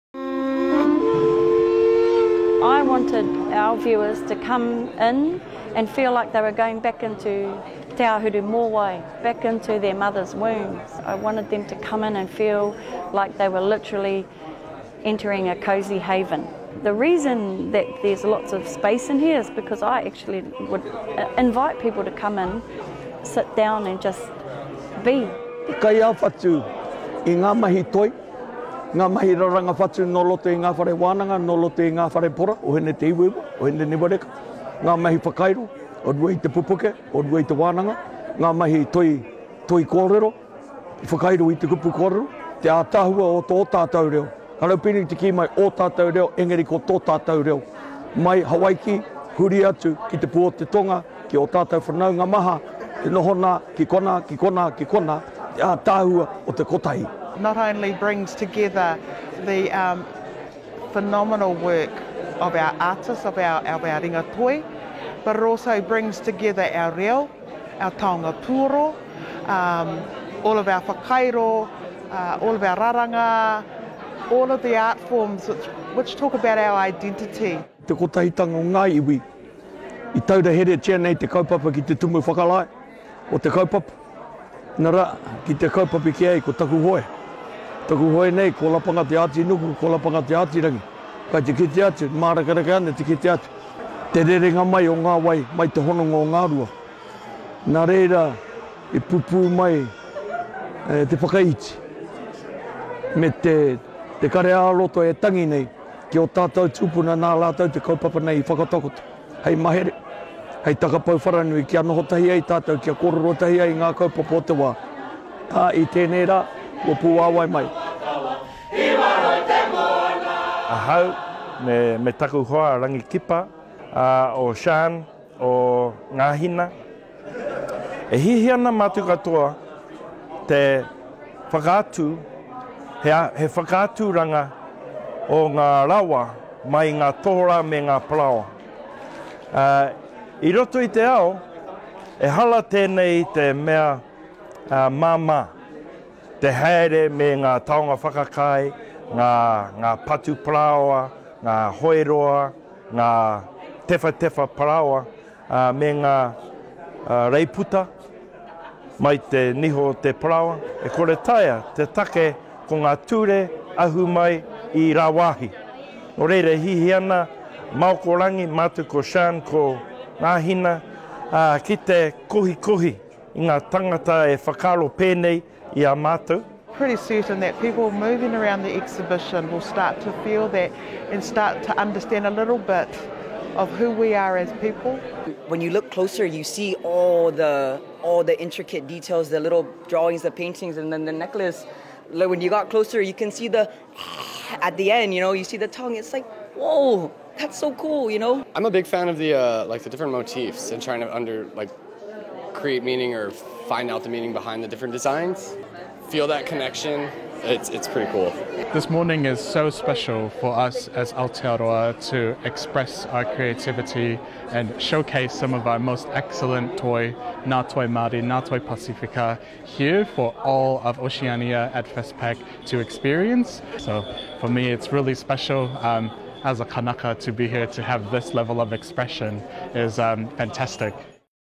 aku Hoe exhibition stirs emotions at FestPAC 2024
After 8 years, New Zealand artists can finally share their work with the Pacific community and the public at FestPAC 2024 in Hawaii. The opening of the New Zealand Taku Hoe art exhibition at the Hawai’i Convention Centre drew over 100 participants, stirring emotions and showing a collective wairua of kotahitanga.